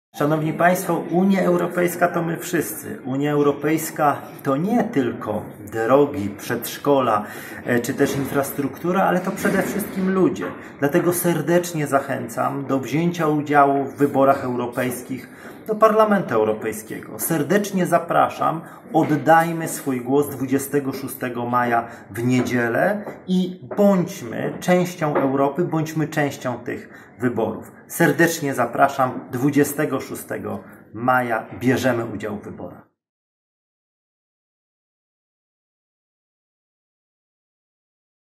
Nie bądź durny, idź do urny! Borys Budka.
Kampania społeczna "Nie bądź durny, idź do urny", która zachęcała mieszkańców Przemyśla do wzięcia udziału w wyborach do Parlamentu Europejskiego w 2019 roku. Wzięli w niej udział znani Przemyślanie i ludzie związani z Przemyślem obecnie bądź w przeszłości. Byli to: Borys Budka, Karol Gajdzik, Anna Grad-Mizgała, Małgorzata Kozaczenko, Wacław Majka, Krzysztof Oczkowski oraz Adam Szostkiewicz. Udział w kampanii wzięły również znane, przemyskie pomniki! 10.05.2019 r.

* produkcja: @studio.jotbe